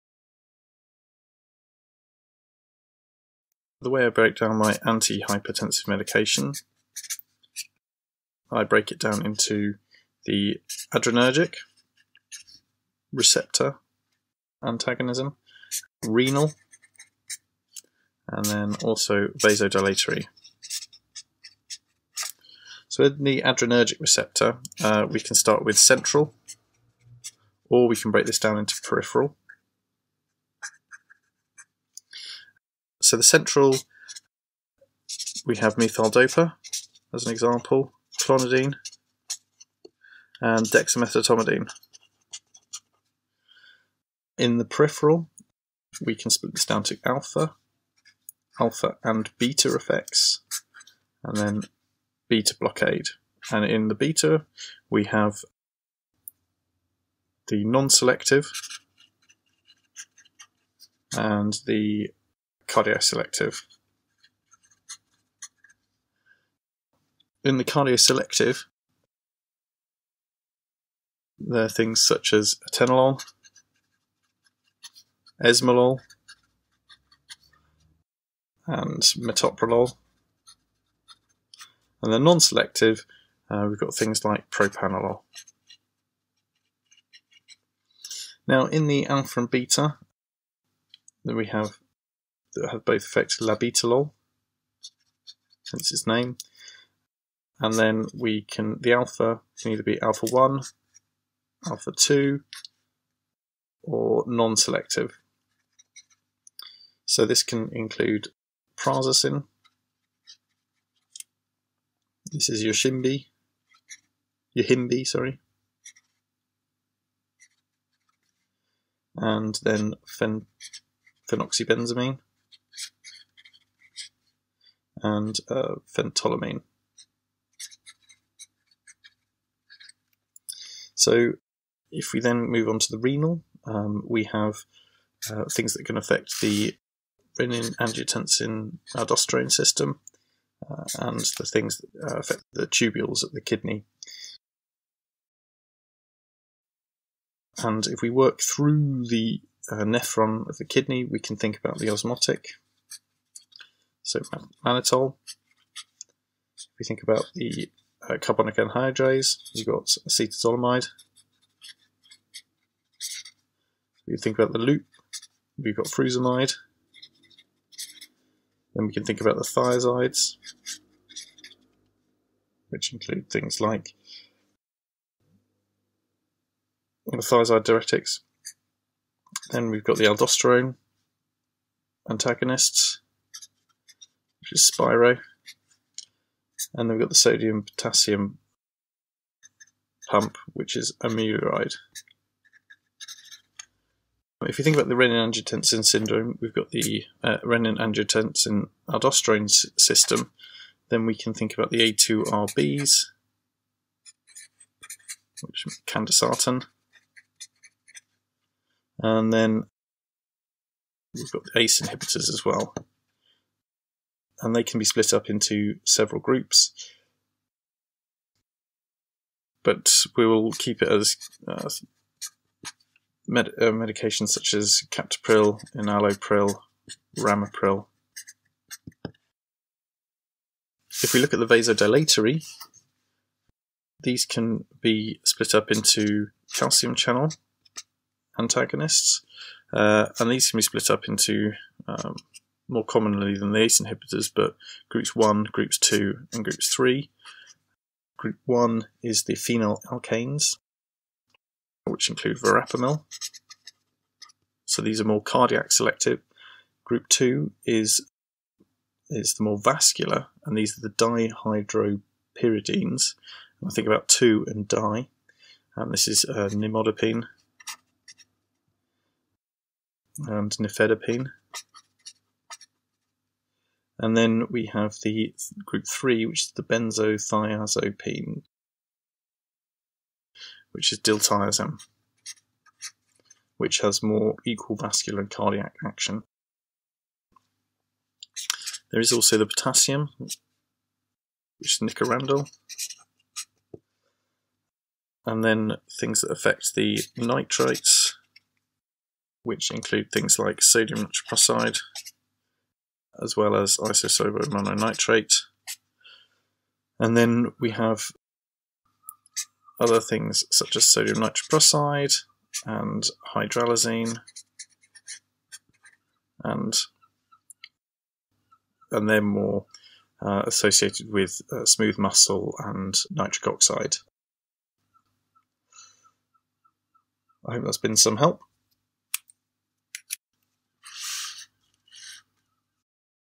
FRCA Revision | Pharmacology | Classification of Anti-hypertensive Medication
A method to breakdown Anti-hypertensive Medication